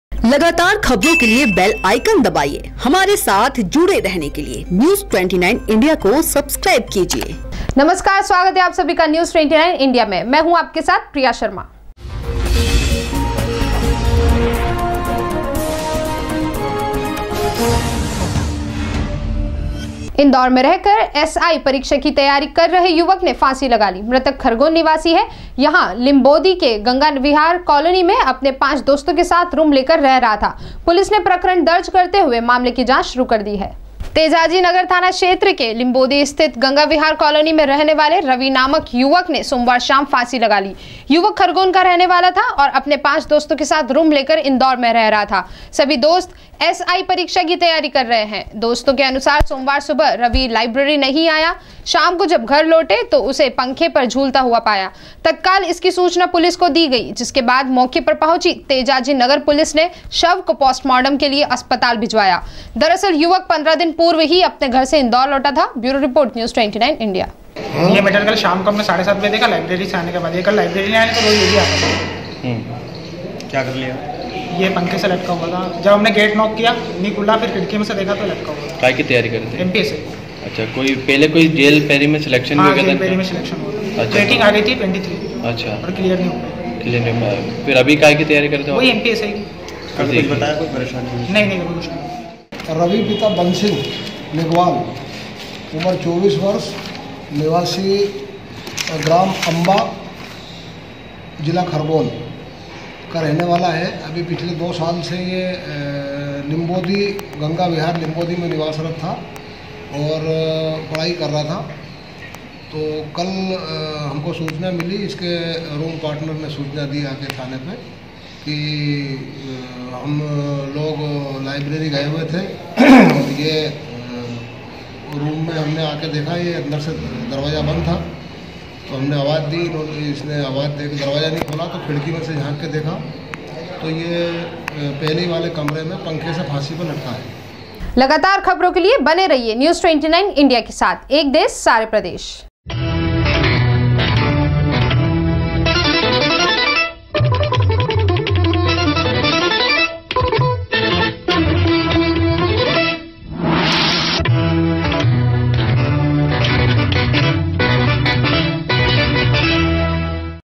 News29India 07 jan #26-एसआई परीक्षा की तैयारी कर रहे युवक ने लगाई फांसी
एसआई परीक्षा की तैयारी कर रहे युवक ने लगाई फांसी
तेजाजी नगर पुलिस ने मामला दर्ज कर जांच की शुरू